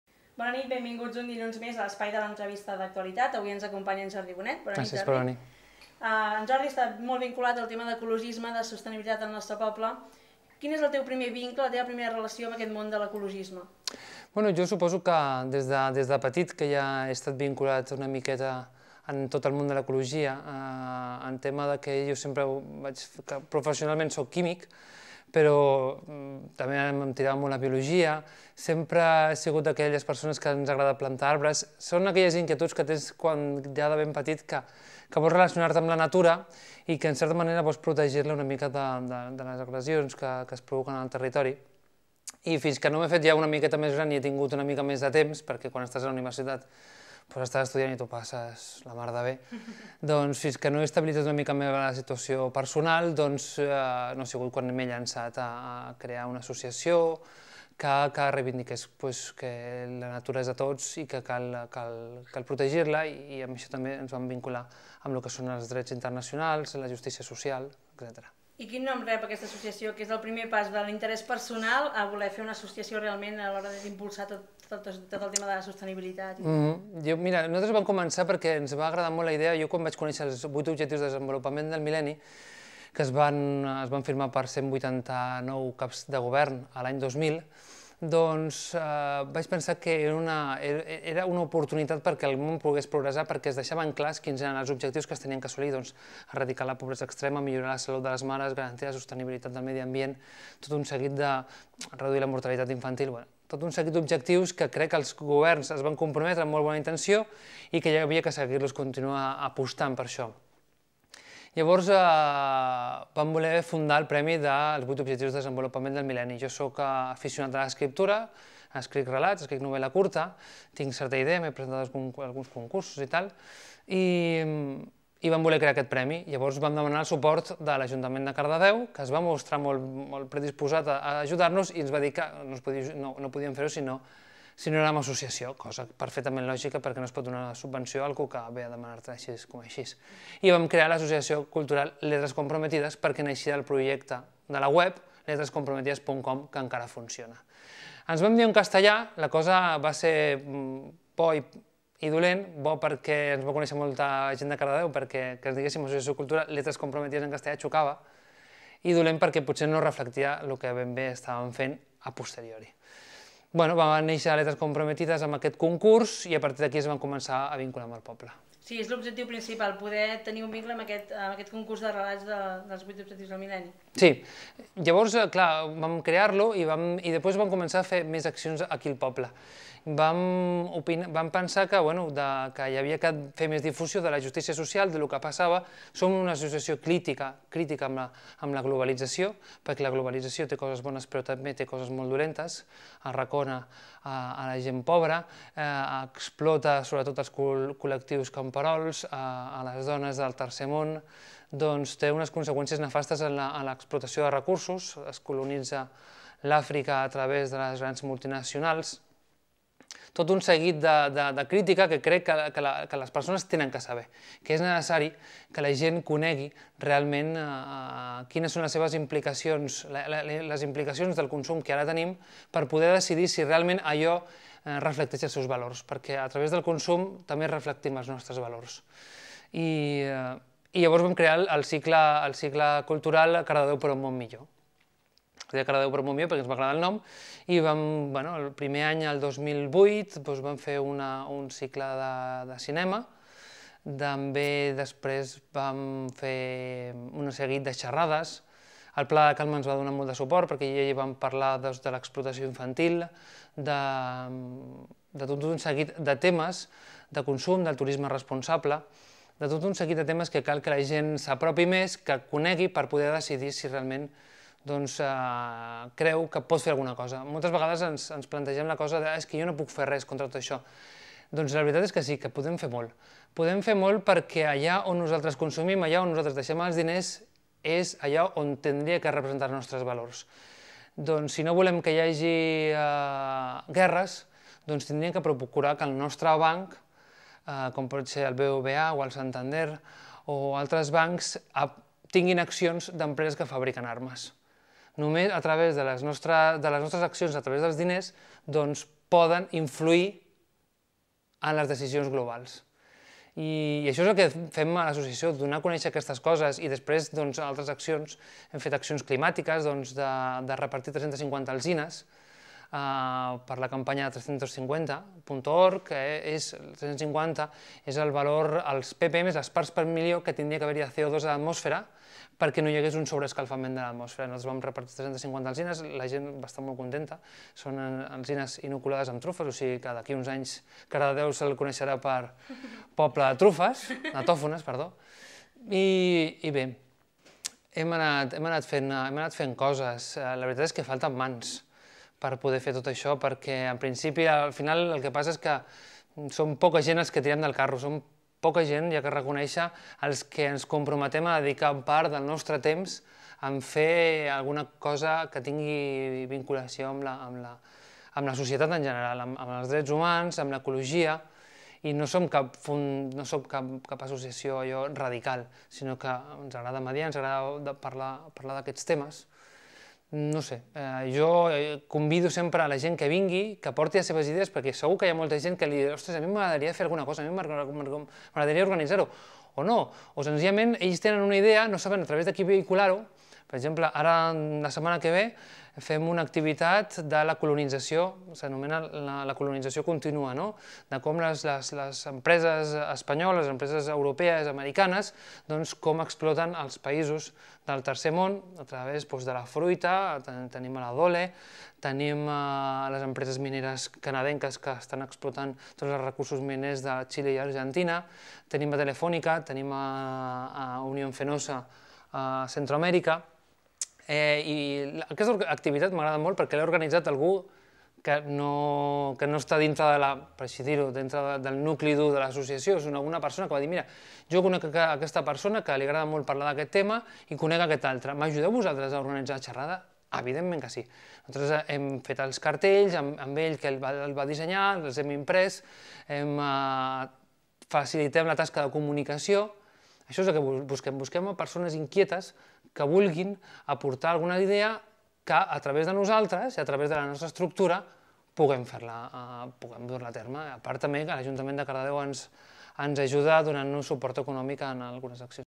L'Entrevista amb Jordi Bonet (1)
Avui entrevistem Jordi Bonet, un dels creadors del web Letras Comprometidas, de l'Associació Cardedeu per un Món Millor i, ara, de la iniciativaCardedeu Autosuficient, una plataforma d'intercanvi dels vegetals conreats als horts urbans de Cardedeu